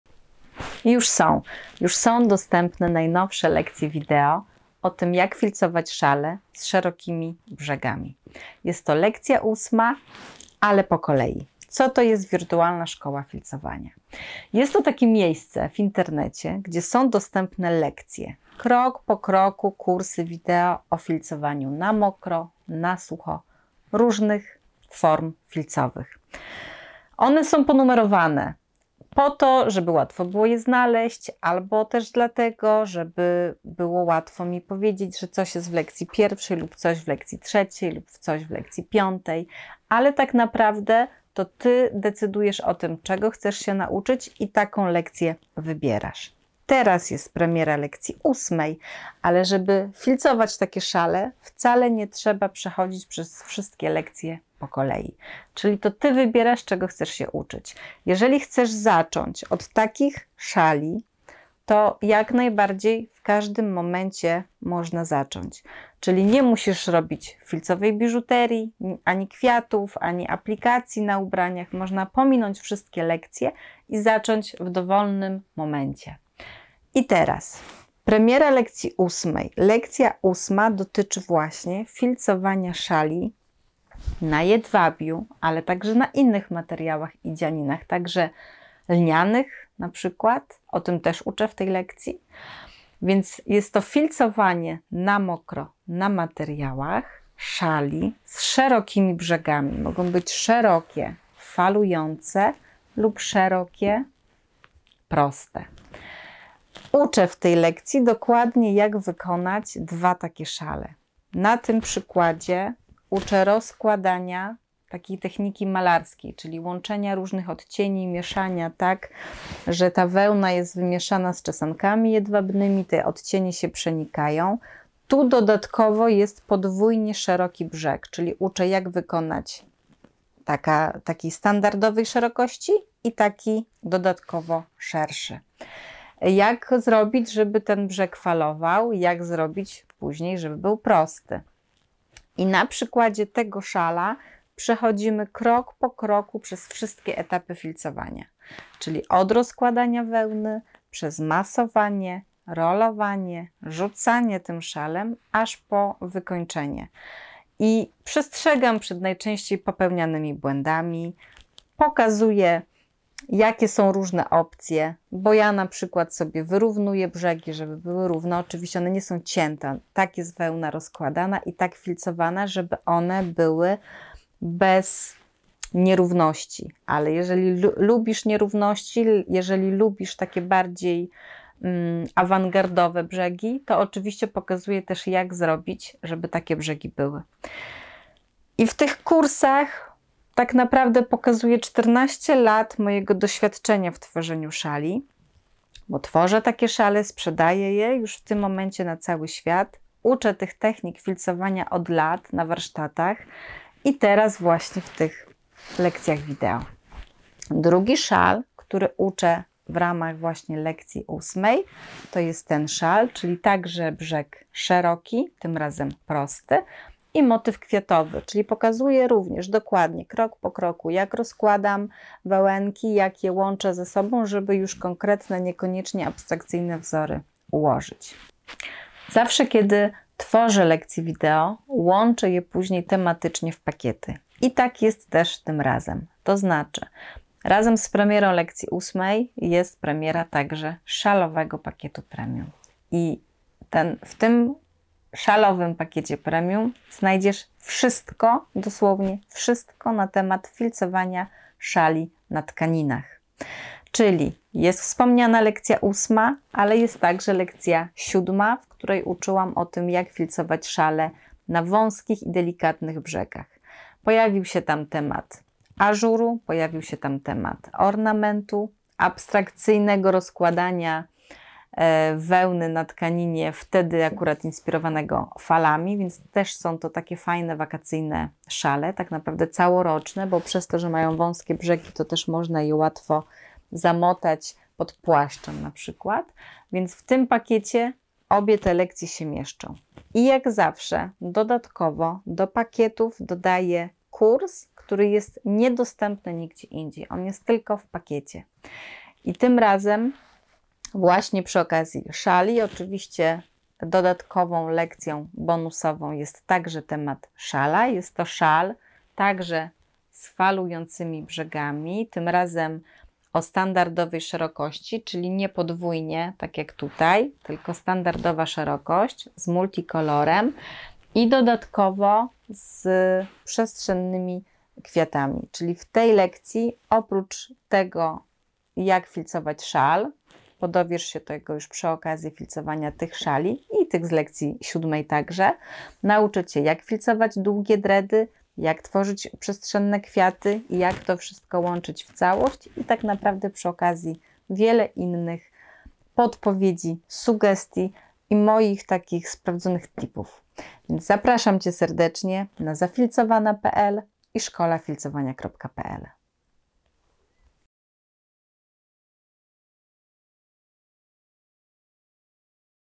Zacznij tworzyć szale jedwabno-filcowe.
Podobają Ci się szale filcowane na jedwabiu? Chciałabyś sama takie szale tworzyć? Dla siebie, na prezenty dla bliskich.. a może na sprzedaż?

Właśnie trwa Premiera kursów wideo o filcowaniu szali na jedwabiu i przez kolejne kilka dni (do końca czerwca) - możesz zdobyć te Lekcje w specjalnych, promocyjnych cenach.